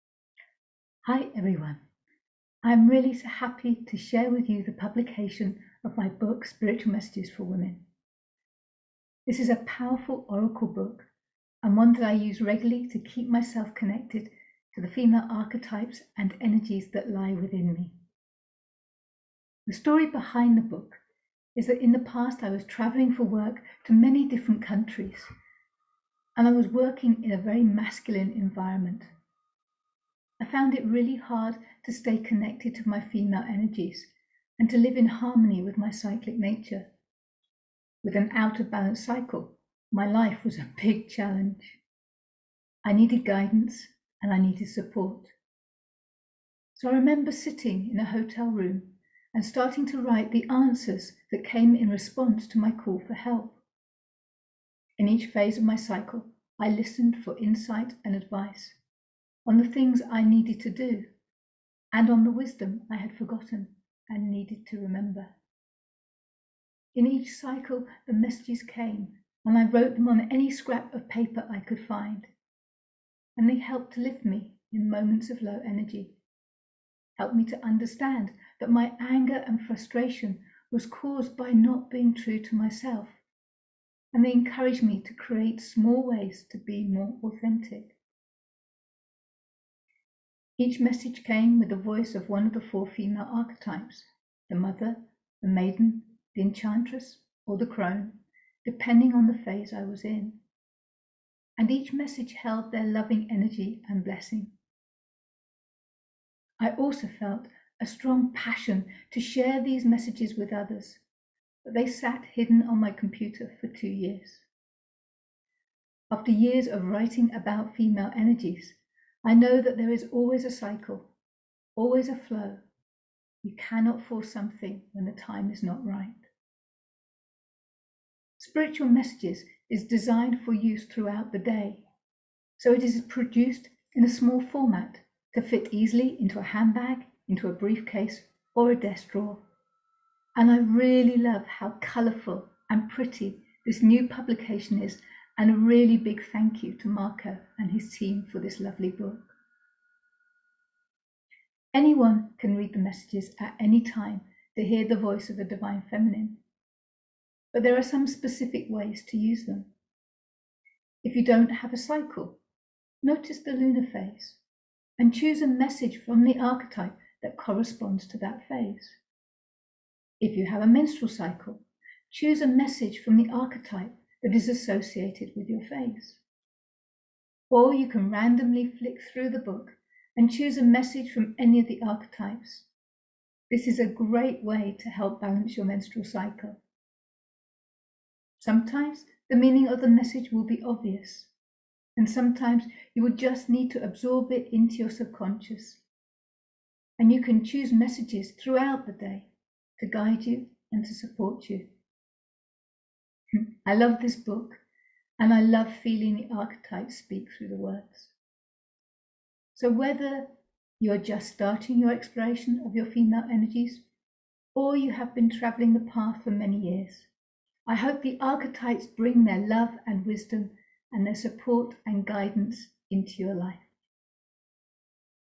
Spiritual Messages for Women - Miranda Gray
Spiritual Messages for Women offers daily inspirational, supportive and loving guidance in tune with your four cycle phases, showing you how to create and enjoy a wonderful loving relationship with the Divine every day throughout the whole month.Dip into the sections once a day or throughout the day to reach out to the Divine and join Her dance.

I'm delighted that 'Spiritual Messages' is now available in an Italian edition as well as Spanish and Czech, and a brand new English edition will be published shortly.